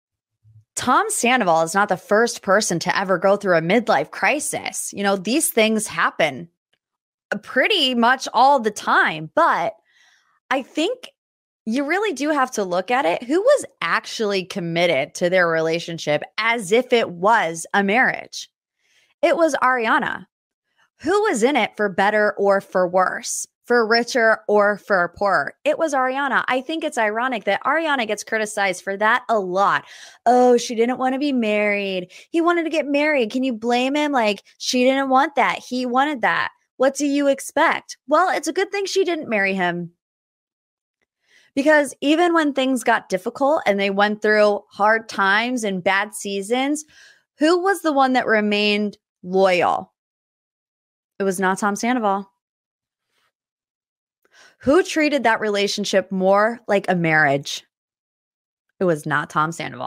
Tom Sandoval's midlife crisis | Morgan's Pop Talks
If you're anything like us you are still reeling from the Vanderpump Rules season 10 finale. Morgan has so much to say she's releasing a bonus episode this week completely dedicated to the episode. Listen tomorrow wherever you get your podcasts or watch on our YouTube channel!